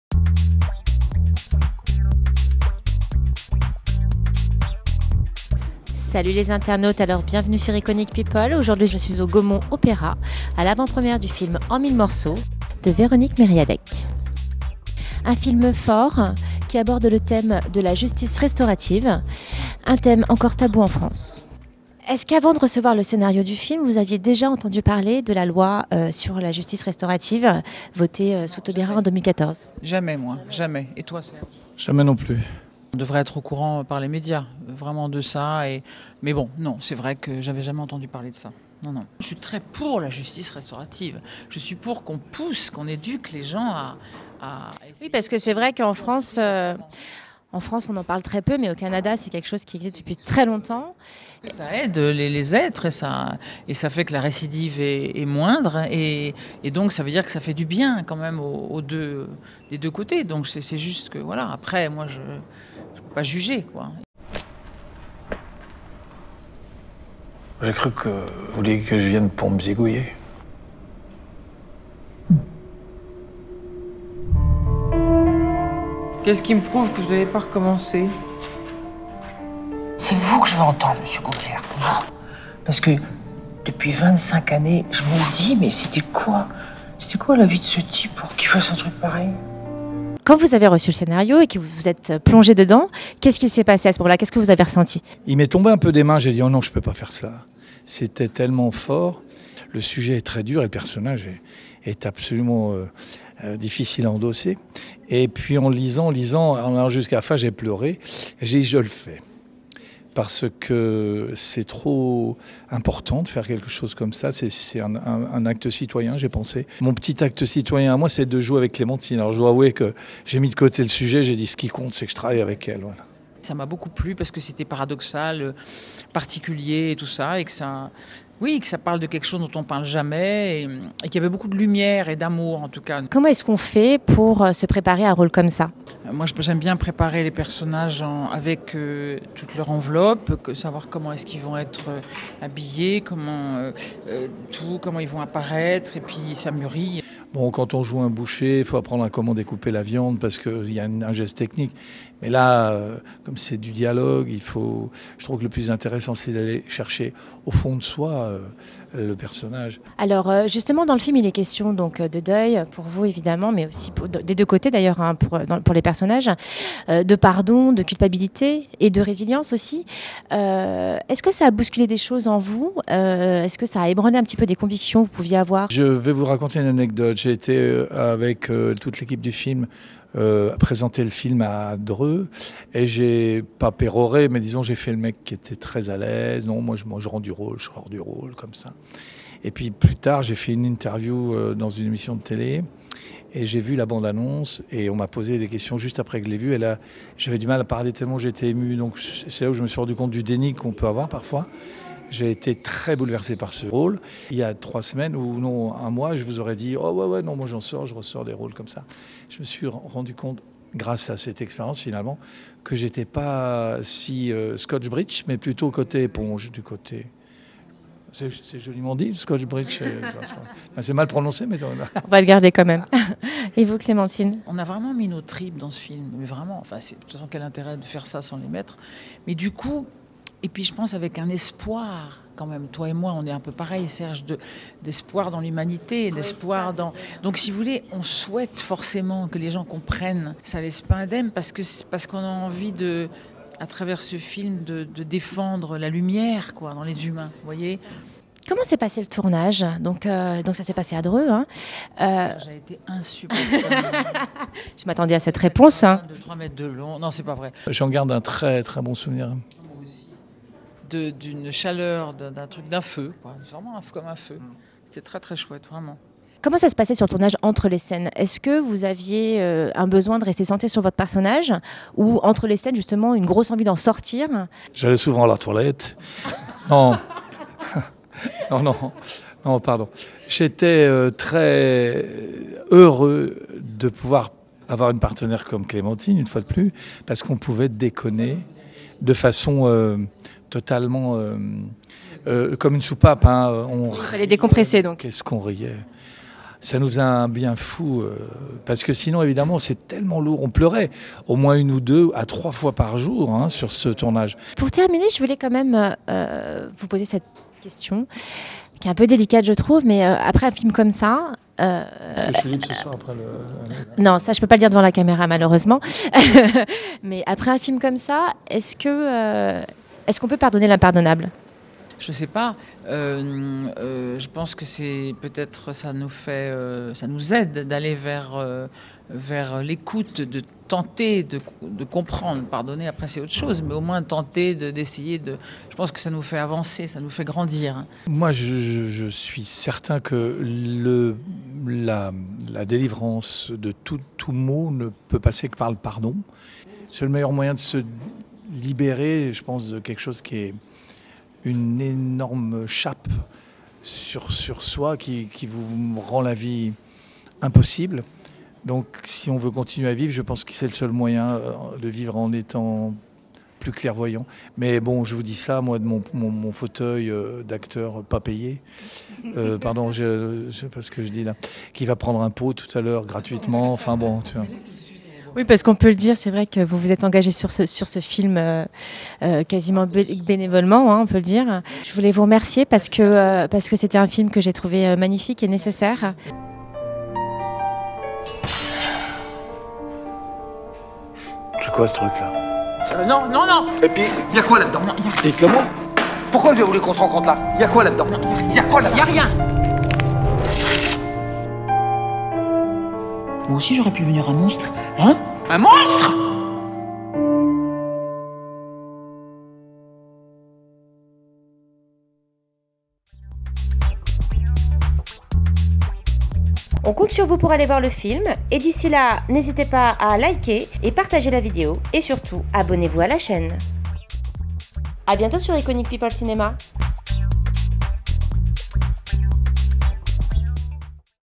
Interview - C.Célarié et S.Riaboukine (En Mille Morceaux)
Véronique Mériadec nous offre ici un huis clos prenant mettant face à face une mère et l'assassin de son fils, tout juste sorti de prison.

Un coup de projecteur nécessaire sur la notion de justice restauratrice dont on ne parle pas assez en France, alors qu'elle est en vigueur depuis 2014 et qu'elle pourrait faire baisser le taux de récidive d'au moins 30% !

Un film nécessaire, dans lequel Clémentine Célarié et Serge Riaboukine se sont engagés bénévolement faute de financement.